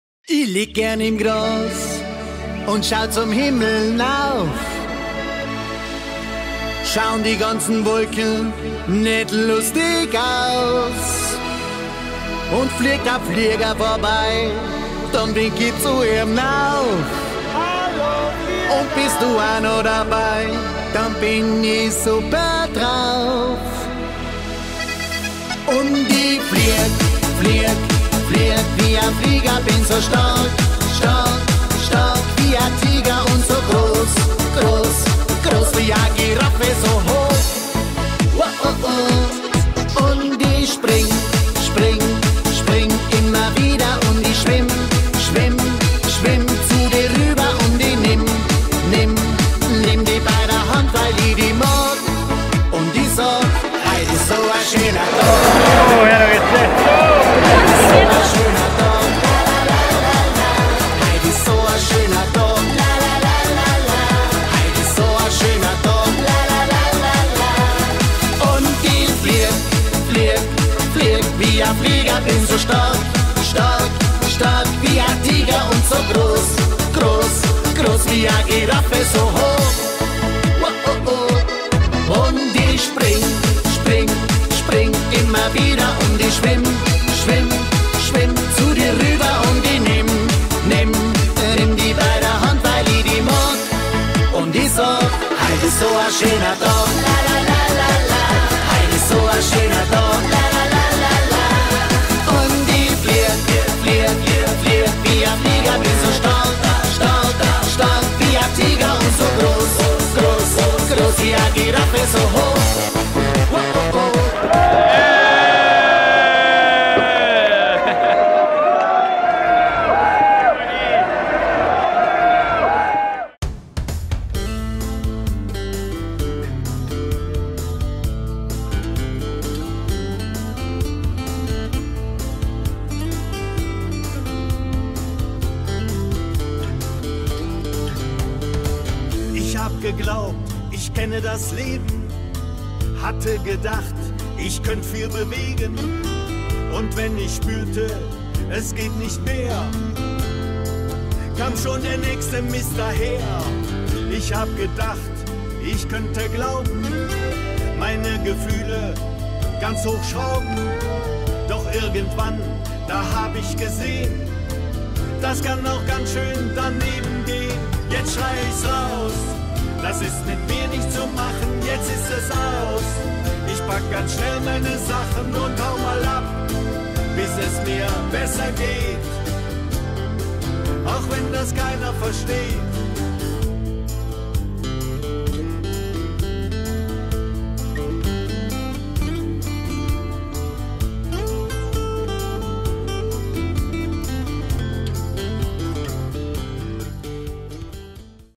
Auf die Wiesn! 🍺 - Oktoberfest 2016
24 de Septiembre 2016
Estando en alemania para fines de septiembre solo puede significar una cosa: Oktoberfest
Fue un encuentro de varios amigos que hace rato que no nos veiamos, compartiendo unas buenas cervezas en el Oktoberfest en Munich.

Todo filmado con una Canon G7X